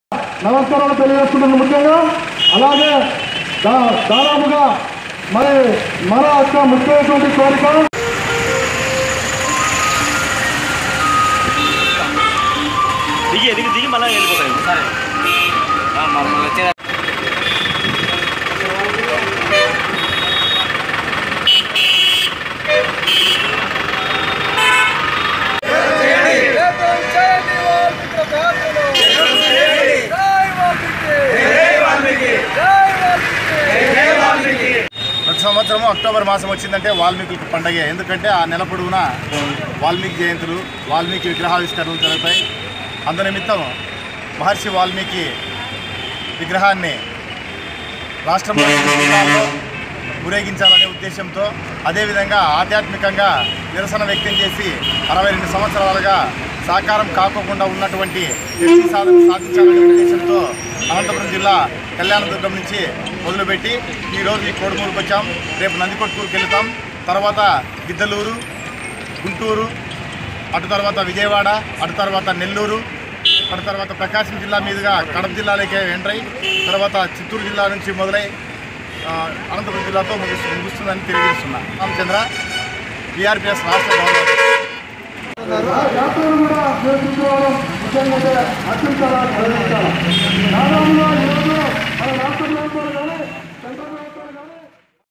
కోడుమూరులో వాల్మీకులను ఎస్టీ జాబితాలో చేర్చాలని కోరుతూ వాల్మీకి రథయాత్ర..
వాల్మీకి మహర్షి రథయాత్ర కర్నూల్ జిల్లా కోడుమూరుకు చేరుకుంది.ఈ కార్యక్రమంలో వాల్మీకి సోదరులు స్వచ్చందంగా పాల్గొని విజయవంతం చేయడం జరిగింది.అనంతపురం జిల్లాలోని కళ్యాణదుర్గంలో ప్రారంభమైన ఈ యాత్ర రాష్ట్రంలోని 8 జిల్లాలో సాగుతుందని తెలిపారు.ఈ యాత్ర డిసెంబర్‌ 8 వరకు సాగుతుందని వాల్మీకి రథయాత్ర నాయకులు పొగాకు రామచంద్రుడు తెలిపారు.వీఆర్‌పీఎస్‌ రాష్ట్ర అధ్యక్షుడు ఎం. సుభాష్‌చంద్రబోస్‌ ,వాల్మీకి రథయాత్ర నాయకులు పొగాకు రామచంద్రుడు ఆధ్వర్యంలో కొడుమురులోని వాల్మీకిలతో కలిసి స్థానిక కొత్త బస్ స్టాండ్ నుండి కోట్ల సర్కిల్ వరకు పాదయాత్రగా వచ్చారు.